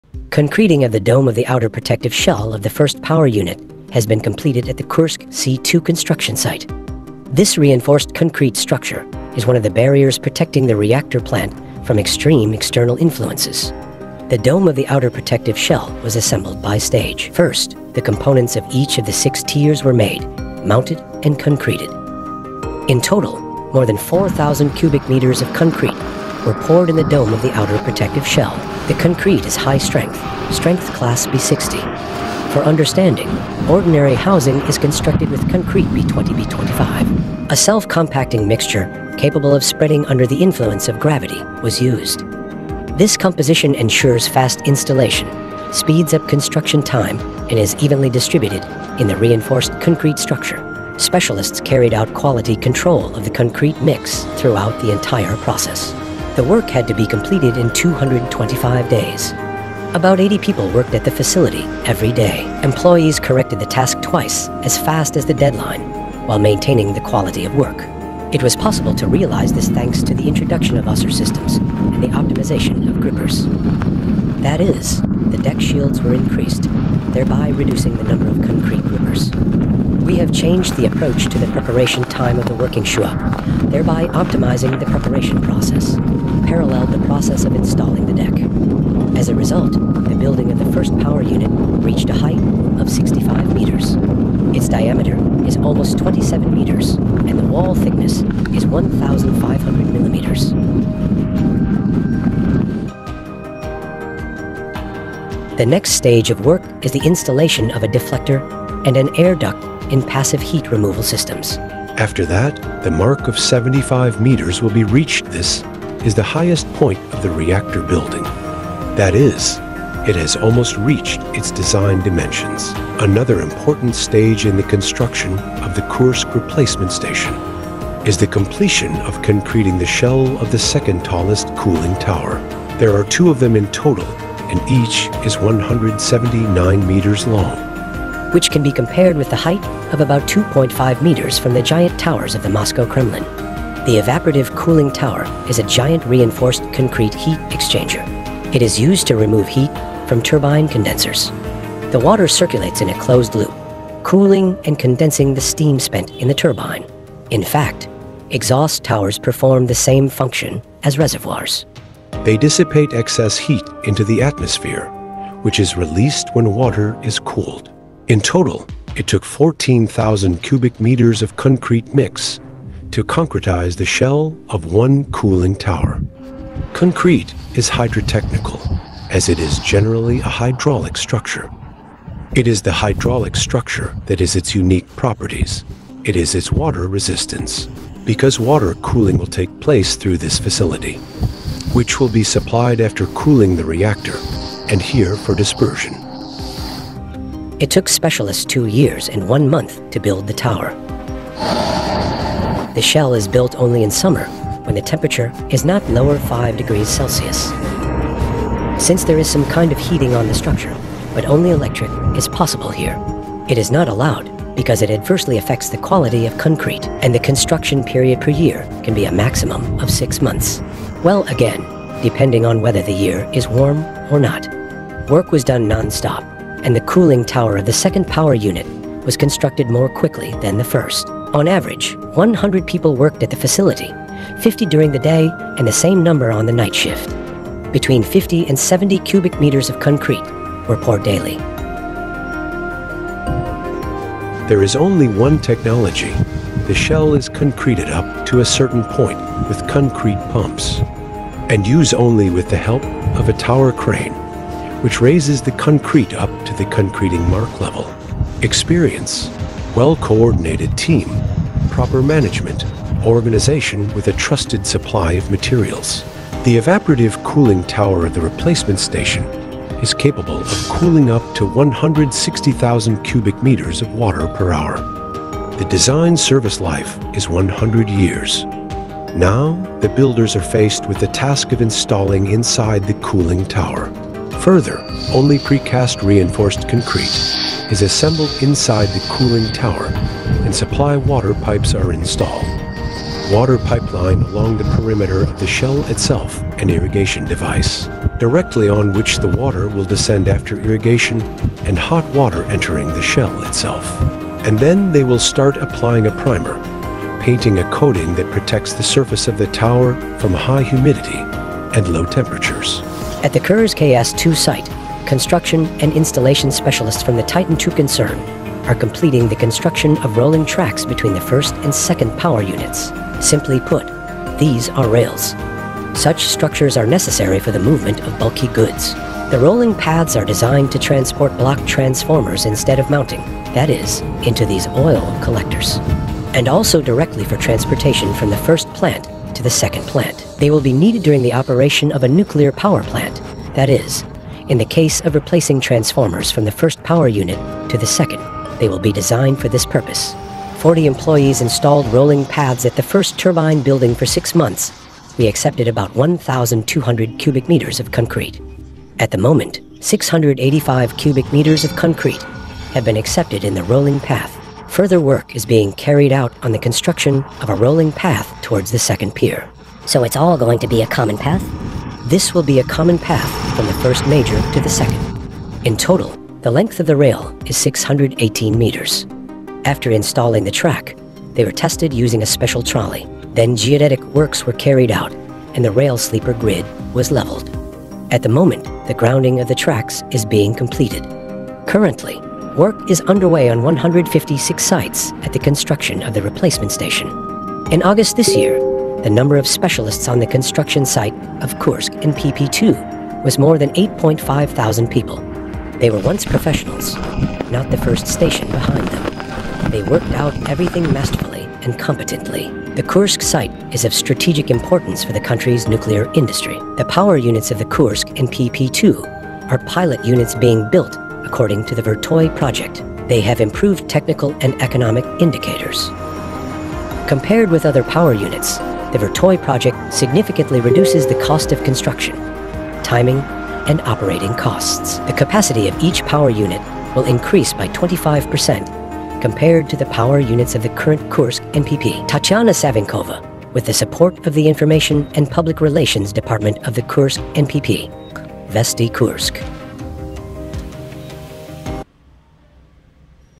Awesome nuclear cooling tower in the world.
welcome to structure expert KH
the Kursk C2 construction site this reinforced concrete structure is one of the barriers protecting the reactor plant from extreme external influences the Dome of the outer protective shell was assembled by stage first the components of each of the six tiers were made mounted and concreted in total more than 4,000 Cub M of concrete were poured in the Dome of the outer protective shell the concrete is high strength class B60 for understanding ordinary housing is constructed with concrete B20 B25 a self- compacting mixture capable of spreading under the influence of gravity was used this composition ensures fast installation speeds up construction time and is evenly distributed in the reinforced concrete structure Specialists carried out quality control of the concrete mix throughout the entire process the work had to be completed in 225 days a about 80 people worked at the facility every day employees corrected the task twice as fast as the deadline while maintaining the quality of work it was possible to realize this thanks to the introduction of user systems and the optimization of grippers that is the deck shields were increased thereby reducing the number of concrete grippers we have changed the approach to the preparation time of the working thereby optimizing the preparation process paralleled the process of installing the deck as a result the building of the first power unit reached a height of 65 M its diameter is almost 27 M and the wall thickness is 1,500
the evaporative cooling tower of the replacement station is capable of cooling up to 160,000 cubic meters of water per hour the design service life is 100 years now the builders are faced with the task of installing inside the cooling tower further only Precast reinforced concrete is assembled inside the cooling tower and Supply water pipes are installed water pipeline along the perimeter of the shell itself an irrigation device directly on which the water will descend after irrigation and hot water entering the shell itself and then they will start applying a primer painting a coating that protects the surface of the Tower from high humidity and low temperatures at the Kur ks2 site construction and installation Specialists from the Titan 2 are completing the construction of rolling tracks between the first and second power units simply put these are rails such structures are necessary for the movement of bulky Goods the rolling paths are designed to transport block Transformers instead of mounting that is into these oil collectors and also directly for transportation from the first plant to the second plant they will be needed during the operation of a nuclear power plant that is in the case of replacing Transformers from the first power unit to the second they will be designed for this purpose 40 employees installed rolling paths at the first turbine building for 6 months we accepted about 1,200 cubic M of concrete at the moment 685 cubic M of concrete have been accepted in the rolling path further work is being carried out on the construction of a rolling path towards the second Pier so it`s all going to be a common path this will be a common path from the first major to the second in total the length of the rail is 618 M after installing the track they were tested using a special trolley then geodetic Works were carried out and the rail sleeper grid was leveled at the moment the grounding of the tracks is being completed currently work is underway on 156 sites at the construction of the replacement station in August this year the number of Specialists on the construction site of Kursk NPP-2 was more than 8. 5 thousand people they were once professionals not the first station behind them they worked out everything masterfully and competently the Kursk site is of strategic importance for the country's nuclear industry the power units of the KK and pp2 are pilot units being built according to the VVRTOI project they have improved Technical and economic indicators compared with other power units the VVRTOI project significantly reduces the cost of construction timing and operating costs the capacity of each power unit will increase by 25% compared to the power units of the current Kursk NPP.